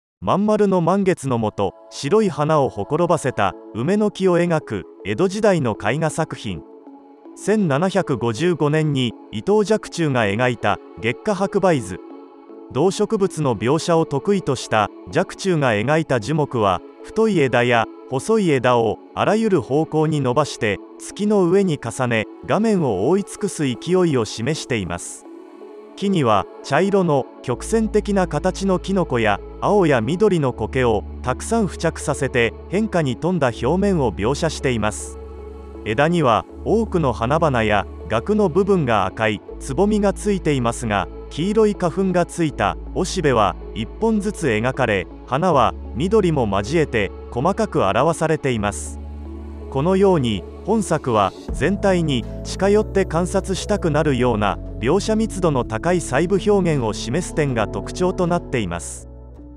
メトロポリタンの名作：日本美術㊼🌼伊藤若冲《月下白梅図》江戸時代 1755年
前回の動画はコチラ➡メトロポリタンの名作：日本美術㊻🌼酒井抱一《鶯白梅図》江戸時代 19世紀初頭

メトロポリタン美術館 の名作を１分で解説するこの動画シリーズ、日本美術 編・第47作目は伊藤若冲《月下白梅図》（江戸時代 1755年）。満月の下に、梅の花や蕾、枝についたきのこや苔まで、大量に、詳しく描いた作品について、細部を拡大しながら詳しく解説します。

その他の「メトロポリタン美術館の名作」はこちらの再生リストからどうぞ。